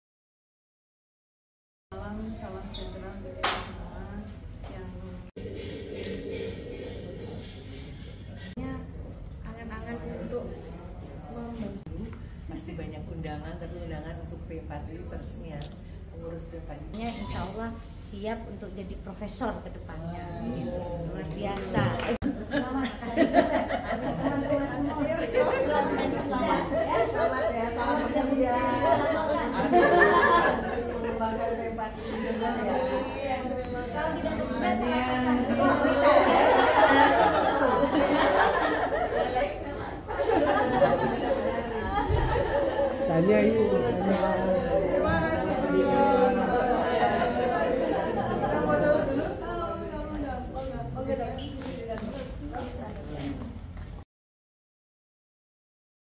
Peresmian dan Pelantikan P4I cabang Jember. Selamat kpd Pengurus P4I Cab.Jember.👍
Pembentukan dan Pelantikan P4I Jember 3 Agustus 2024
Ketua : Dr.dr.Yunita Sp.ParK
dihari oleh Dekan FK Univ Jember Dr.dr.Ulfa Elfianti Sp BP -RE
dan disaksikan oleh Prof.Zailina dan Ptof.Jamal ahli Kesling dr UzpM- University Putra Malaysia

Semula Jember ikut P4I Surabaya.( Unair)
Di Jatim ada juga  P4I Malang ( Univ Brawijaya

Bravo P4I